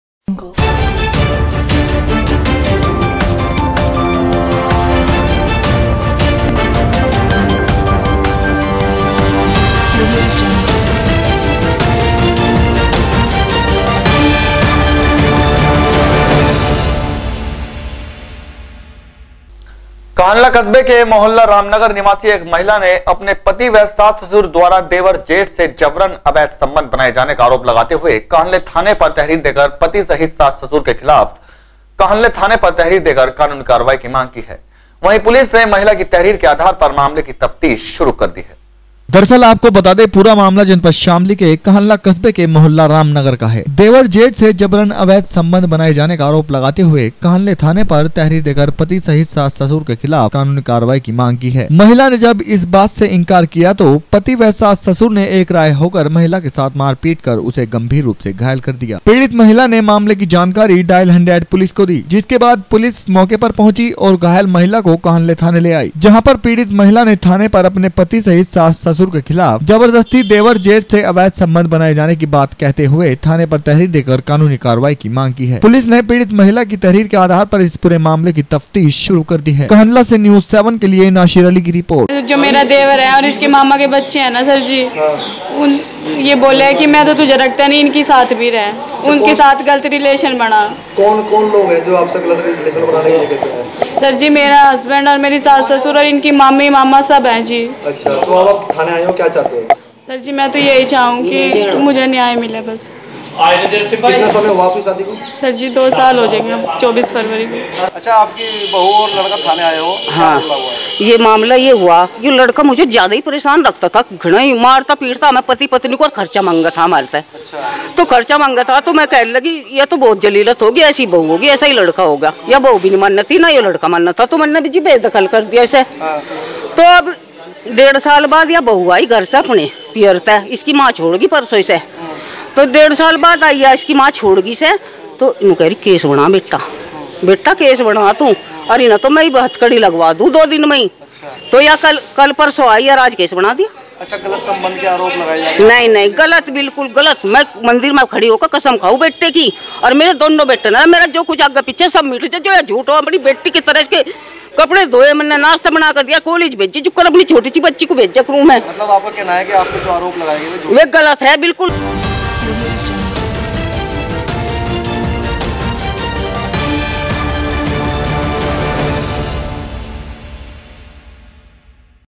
22 Feb 2018 News7Tv- देवर र्जेंट से अवैध संबंध बनाने से इनकार किया तो कर दिया घायल
शामली-कांधला कस्बे के मोहल्ला रामनगर निवासी एक महिला ने अपने पति व सास ससुर द्वारा देवर जेट से जबरन अवैध संबंध बनाए जाने का आरोप लगाते हुए कांधला थाने पर तहरीर देकर पति सहित सास-ससुर के खिलाफ कांधला थाने पर तहरीर देकर कानूनी कार्यवाही की मांग की है। वही पुलिस ने महिला की तहरीर के आधार पर मामले की तफ्तीश शुरू कर दी है।
-दरअसल आपको बता दें पूरा मामला जनपद शामली के कांधला कस्बे के मोहल्ला रामनगर का है। जहां पर एक महिला ने अपने पति सहित सास-ससुर पर द्वारा देवर जेट से जबरन अवैध संबंध बनाए जाने का आरोप लगाते हुए कांधला थाने पर तहरीर देकर पति सहित सास-ससुर के खिलाफ कांधला थाने पर तहरीर देकर कानूनी कार्यवाही की मांग की है। महिला ने जब इस बात से इनकार किया तो पति व सास ससुर ने एक राय होकर महिला के साथ मारपीट कर उसे गंभीर रूप से घायल कर दिया महिला पीड़िता ने मामले की जानकारी डायल हंड्रेड पुलिस को दी जिसके बाद सूचना पर पहुंची पुलिस ने घायल महिला को कांधला थाने ले आई जहां पर पीड़िता महिला ने थाने पर अपने पति सहित सास-ससुर के खिलाफ जबरदस्ती देव र्जेंट से अवैध संबंध बनाए जाने की बात कहते हुए थाने पर तहरीर देकर कानूनी कार्यवाही की मांग की है पुलिस ने पीड़ित की तहरीर के आधार पर इस पूरे मामले की तफ्तीश शुरू कर दी है।